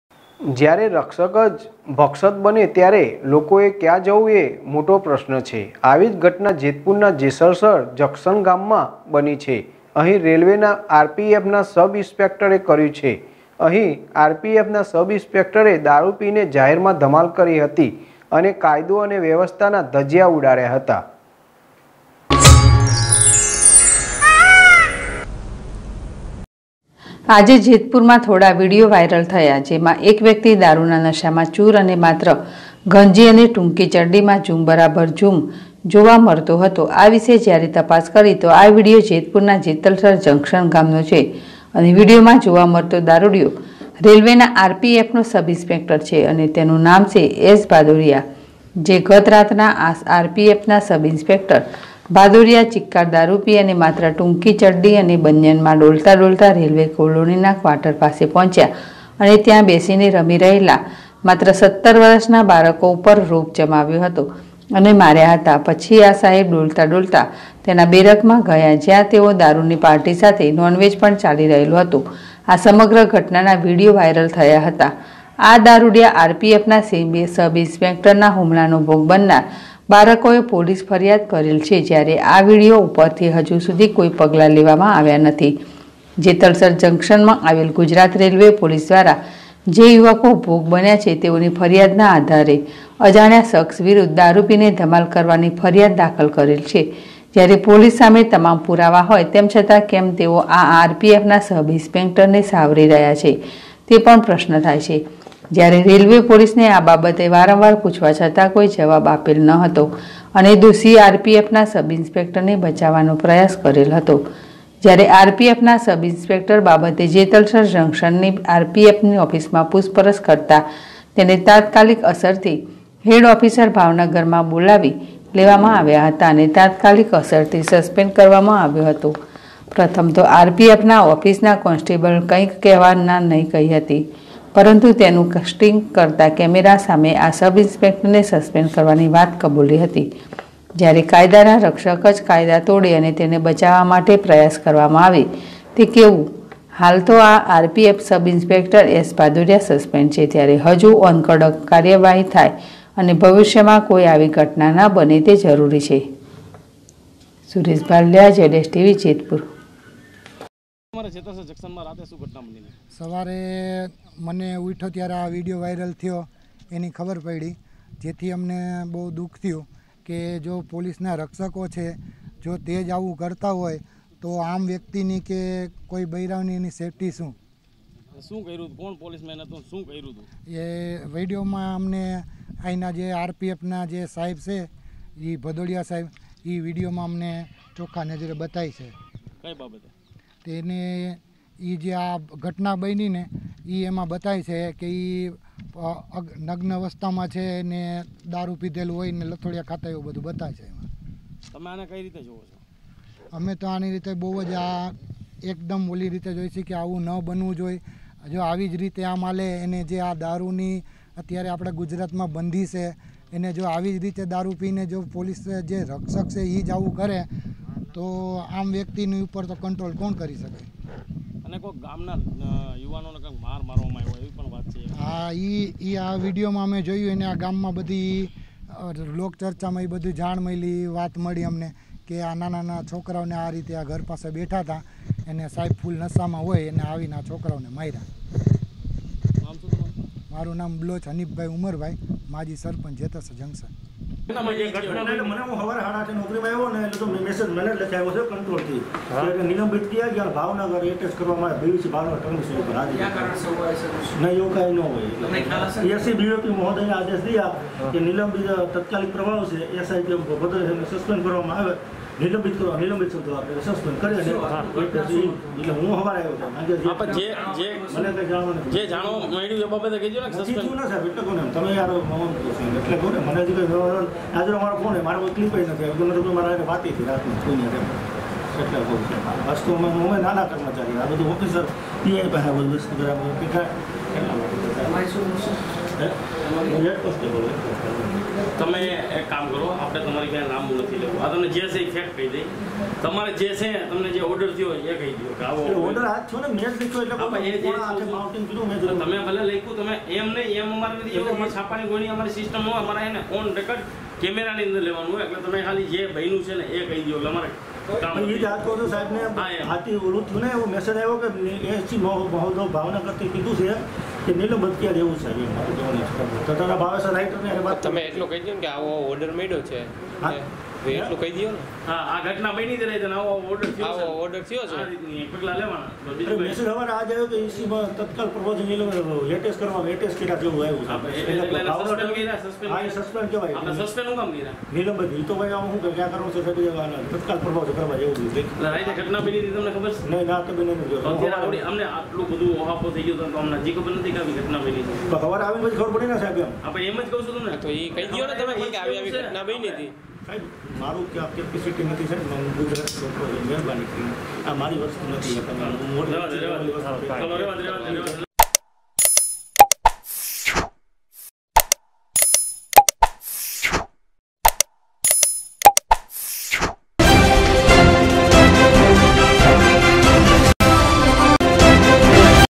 જેતપુર ના જેતલસર જંકશન ગામ માં બની છે, અહીં રેલવે ના RPF ના સબ ઇન્સ્પેકટરે કયું છે
જેતપુર જ્યારે રક્ષક જ ભક્ષક બને ત્યારે લોકો એ ક્યાં જવું તે મોટો પ્રશ્ન છે.